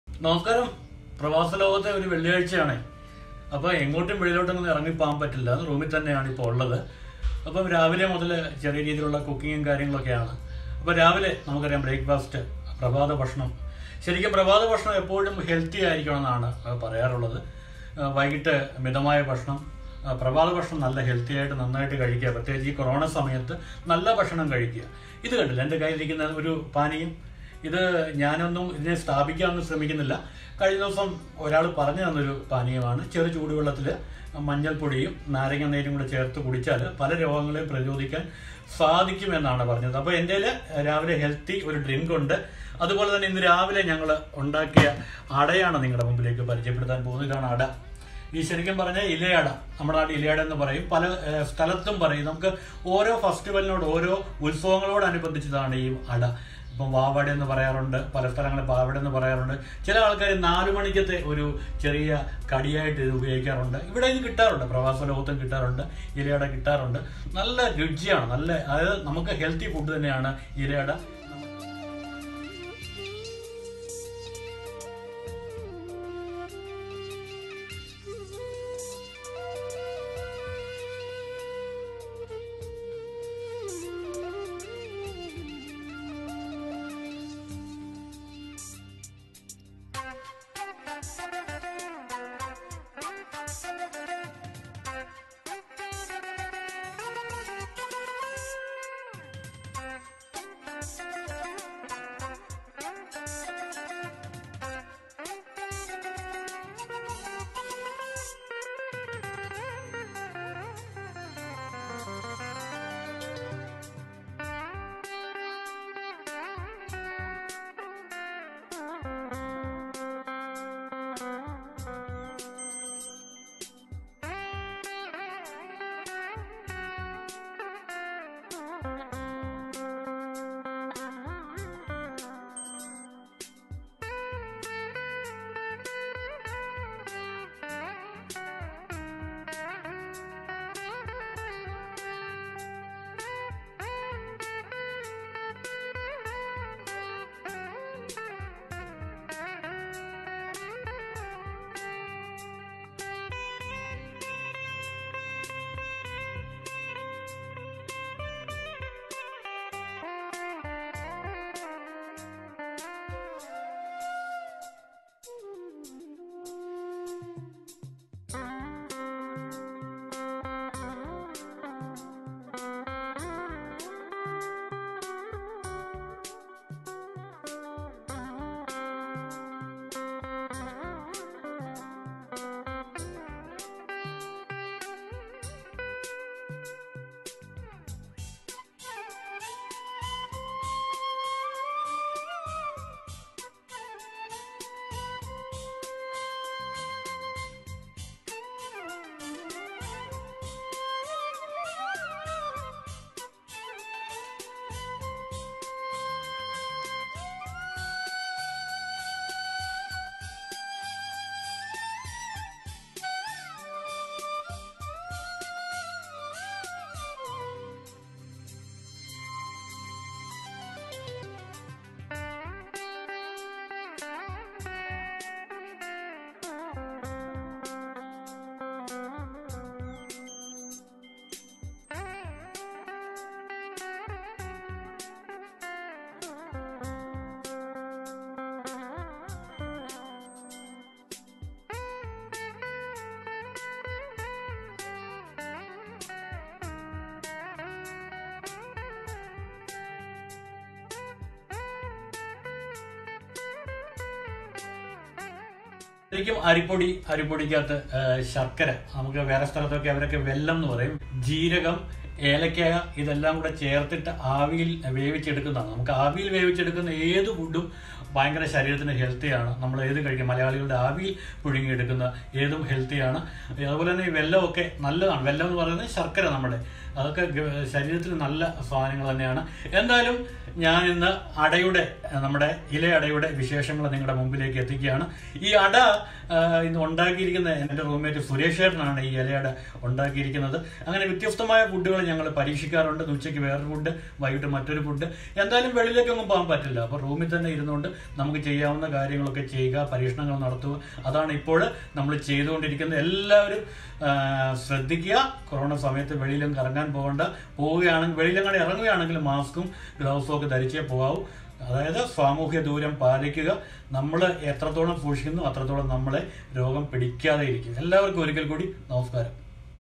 കൊറോണ കാലത്ത് രോഗ പ്രതിരോധ ശേഷി വർദ്ധിപ്പിക്കുന്ന ഹെൽത്തി ഡ്രിങ്കും, വെല്ലം അടയും
ഹെൽത്തി ഡ്രിങ്ക് പെട്ടെന്ന് തയ്യാറാക്കാം...